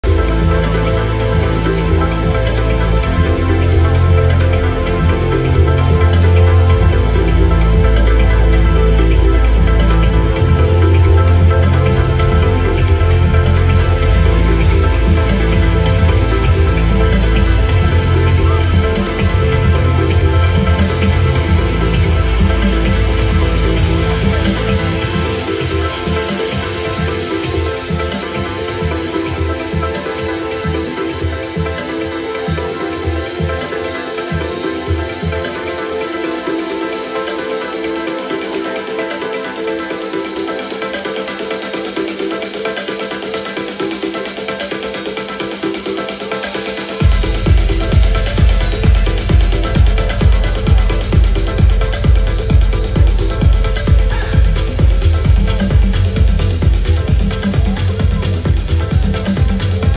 Henry Saiz live in Argentina rosario
Short of live músic festivals. Once a week a Mix of Progressive House and Melodic Techno with new music of my favorite labels and artists. From Rosario, Argentina. Ambient music with seamless transitions.

Visual VJ loops HD artistic visuals with tripy lsd content. Relaxing and psicodelic visual for the music.